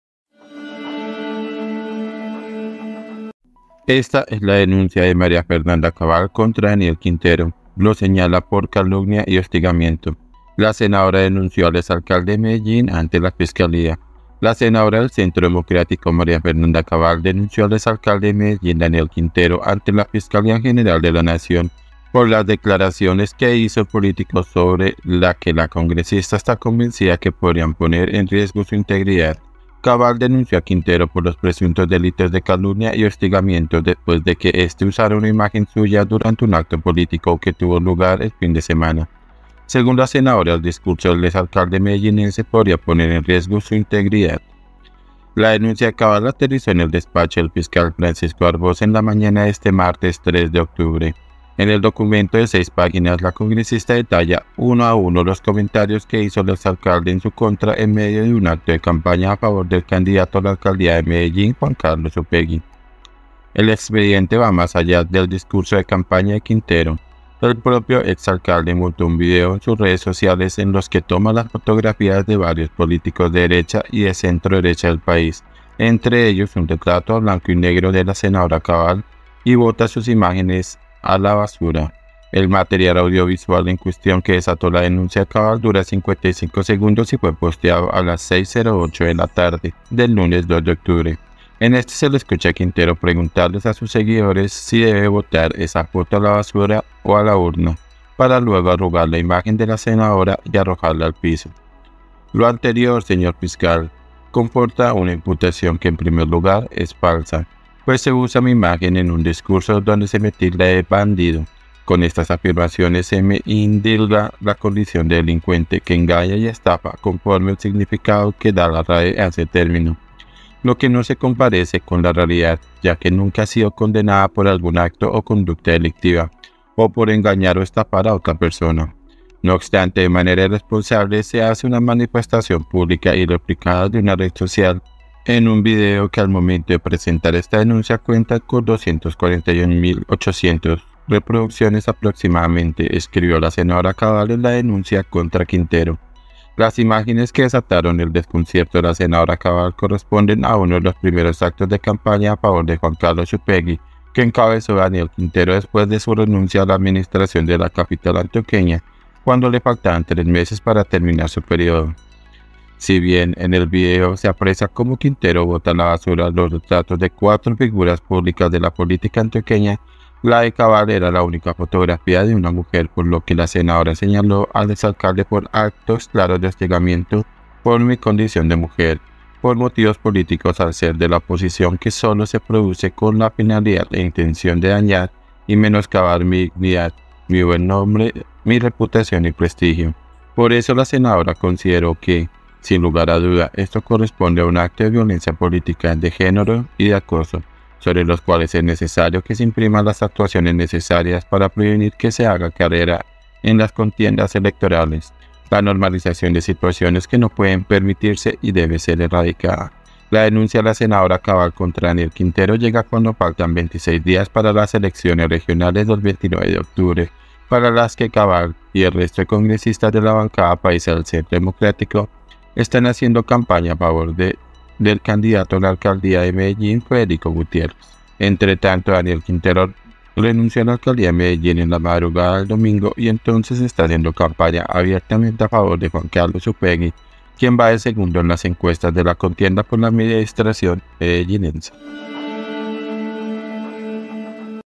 LOÚLTIMO 🔴 MARIA FERNANDA CABAL DENUNCIA A DANIEL QUINTERO ANTE LA FISCALÍA
La senadora del Centro Democrático, María Fernanda Cabal, denunció al exalcalde de Medellín, Daniel Quintero, ante la Fiscalía General de la Nación por las declaraciones que hizo el político sobre las que la congresista está convencida que podrían poner en riesgo su integridad.
FUENTE SEMANA